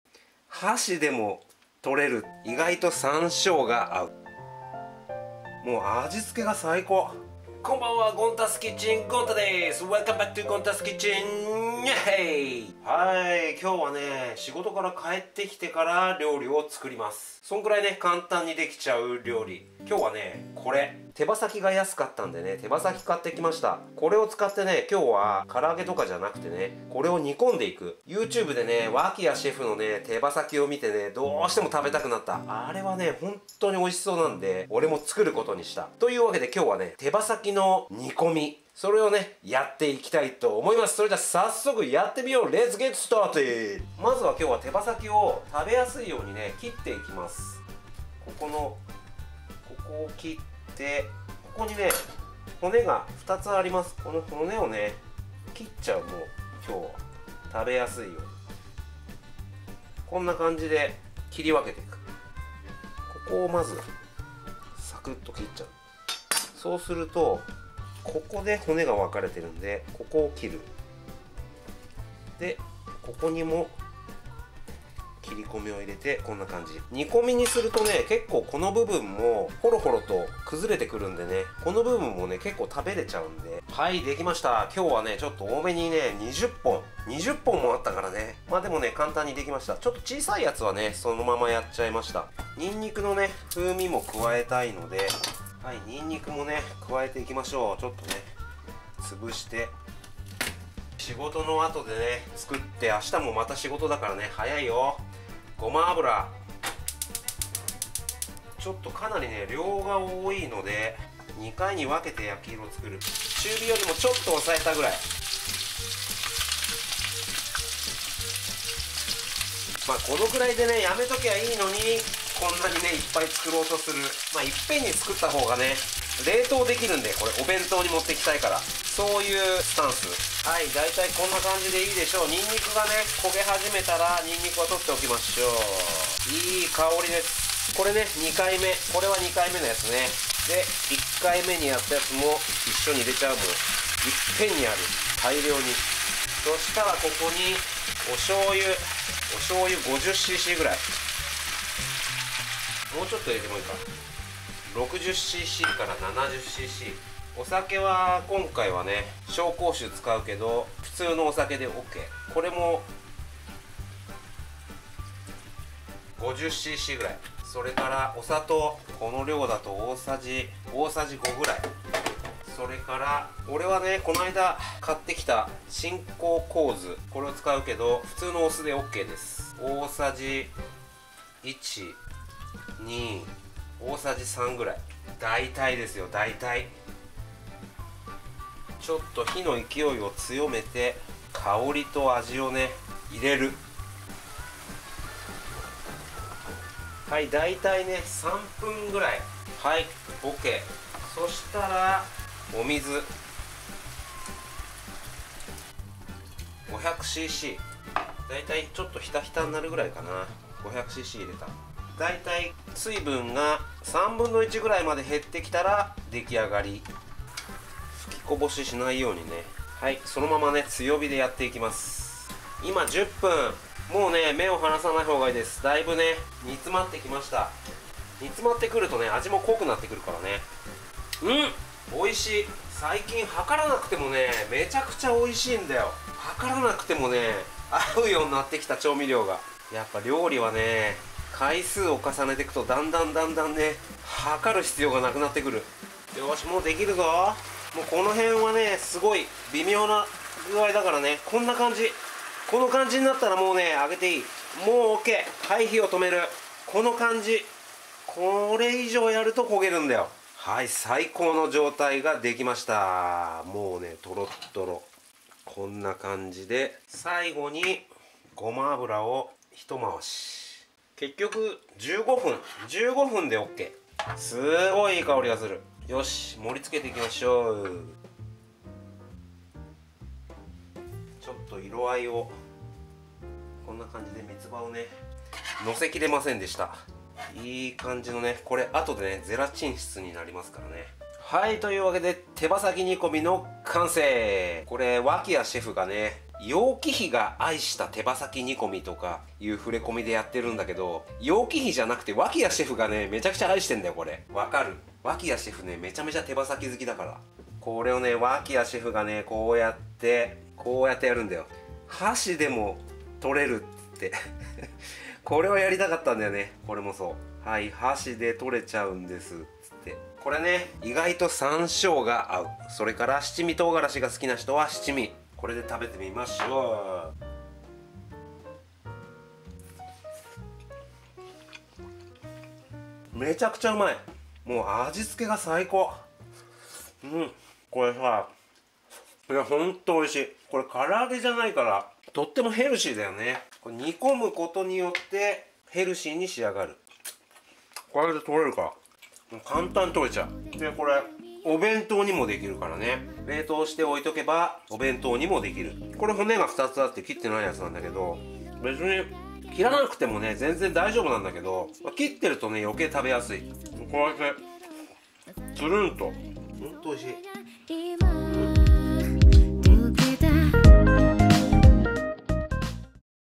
Braised Chicken Wings Japanese style Recipe
Soft & Tender Chicken wings Easy recipe you can even cook in weekdays. Japanese soysauce based seasoning. Delicous!

What you need　（For 3～5)
〇　Chicken wings　　　　　　　　20
〇　Garlic　　　　　　　　　　  　 2~3 cloves
〇　Soysauce　　　　　　　　　　70㏄
〇　Sake　　　　　　　　　　　　 50㏄
〇　Vinegar　　　　　　　　　　　3 tbsp
〇　Sugar　　　　　　　　　　   　5 tbsp
〇　Sesame oil　　　　　　　　　  1 tbsp
〇　Water                                                500cc

〇　Any herbs your like for toppings I used Mitsuba herb
〇　Sansho or Shichimi Japanese pepper matches
〇　I used Chinese Shaoxing wine for Sake
〇　I used Zhen Jiang vinegar for vinegar

手羽先煮の作り方 肉がホロホロで食べやすい！

仕事帰りで作れるくらい簡単なので手羽先が安かったら是非作ってみてください。食べやすいように作る前に切り分けました。冷凍保存もできるので大量につくっても大丈夫。お弁当に最適、簡単なのにめちゃくちゃ美味しくて手が止まりません。子供も大好き家族大絶賛のレシピです。

〇　手羽先　　　　　　　　　　　    　20本
〇　にんにく　　　　　　　　　　  　  2~3 片
〇　醤油　　　　　　　　　　　　　　70㏄
〇　酒　　　　　　　　　　　　 　　　50㏄
〇　酢　　　　　　　　　　　　　  　大さじ 3
〇　砂糖　　　　　　　　　　   　       大さじ 5
〇　ごま油　　　　　　　　　              大さじ１
〇　水                                                             500cc

〇　最後に三つ葉を乗せました
〇　山椒が合います
〇　紹興酒で中華風にしました普通の酒でOK
〇　鎮江香酢で中華風にしました普通の酢でOK

質問やコメント喜んで！
次の動画をつくる励みになります。皆様の応援がエネルギーになり、動画を作成しています